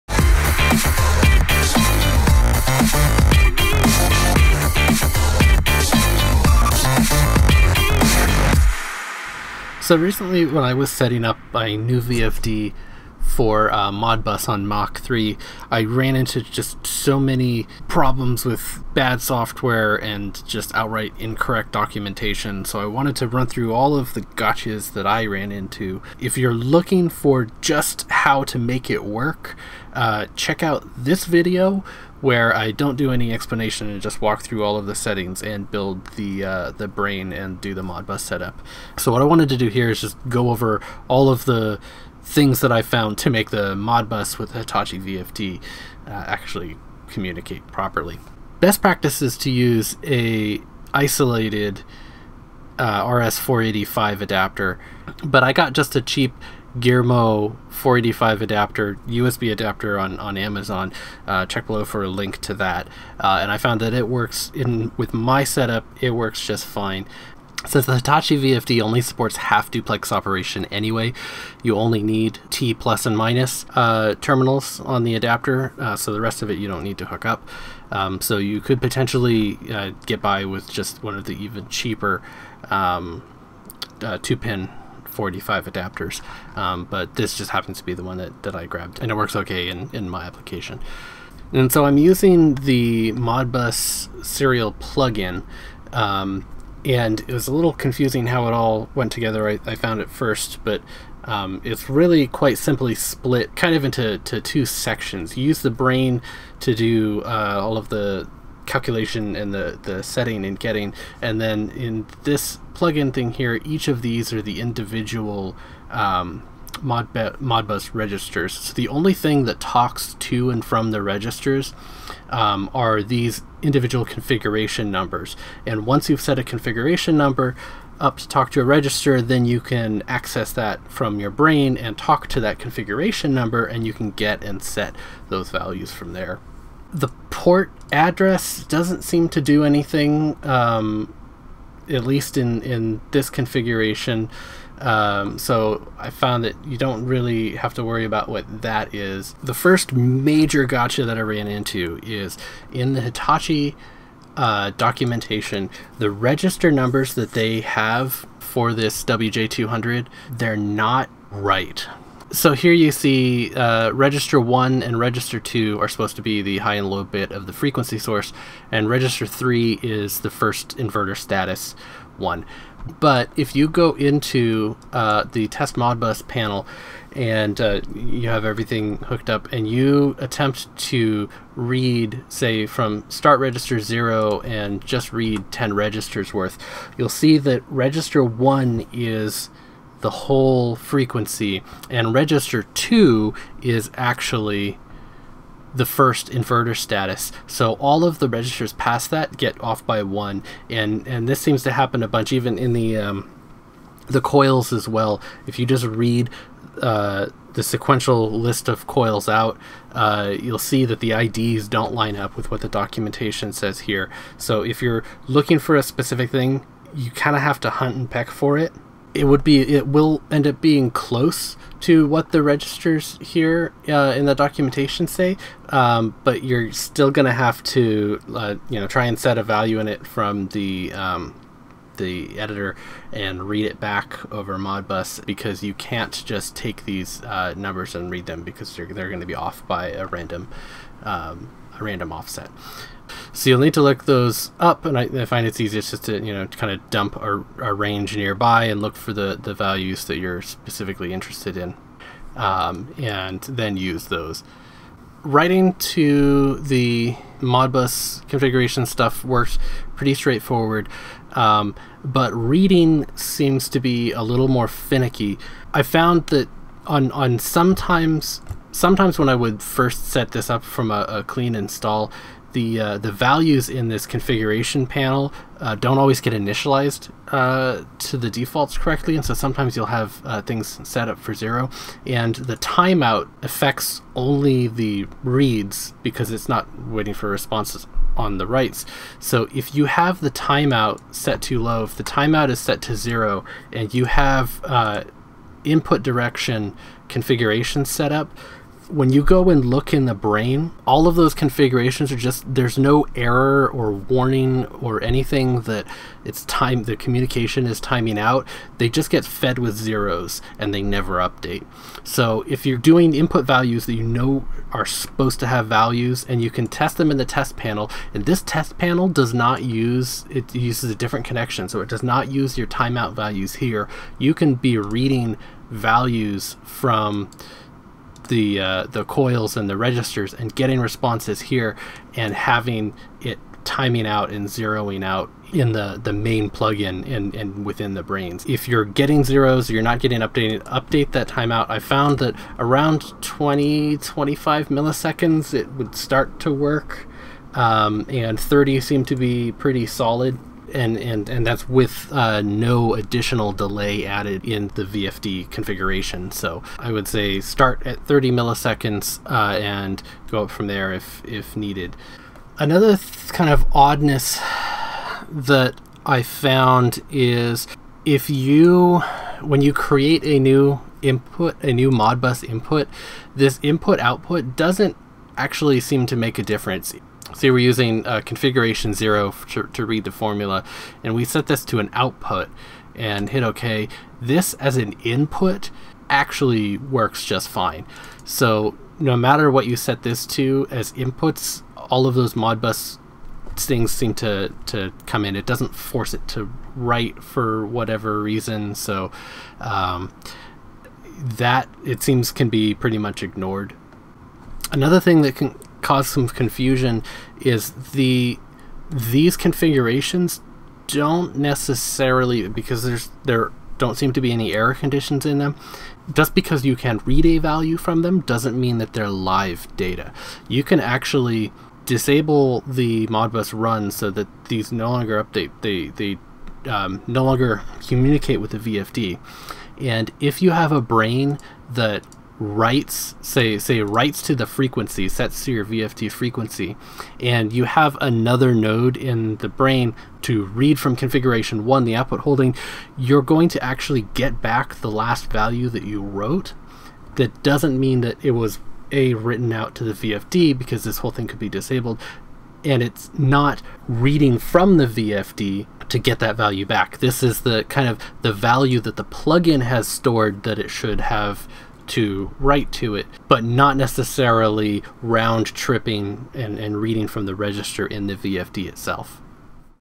WJ200 - Mach3 Modbus troubleshooting
Going over all the gotchas I ran into setting up the VFD

Link for the RS485 adapter used: